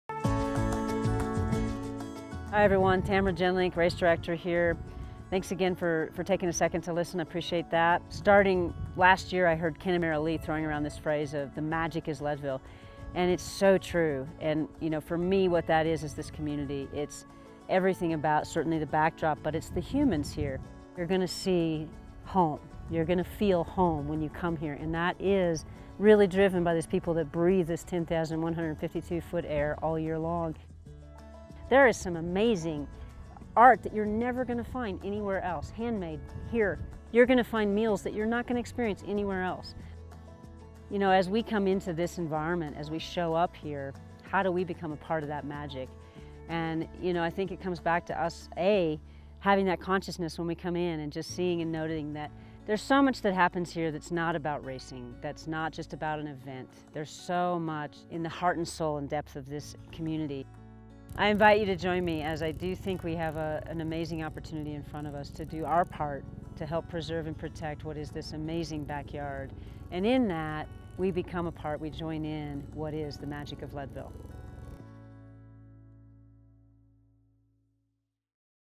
Explore & Protect Leadville - Twin Lakes: Leadville Race Series
Learn more about "The Magic of Leadville" from our Race Director, Tamira Jenlink. We can't wait to see you all and hope you get to explore and help protect the magic that is Leadville - Twin Lakes!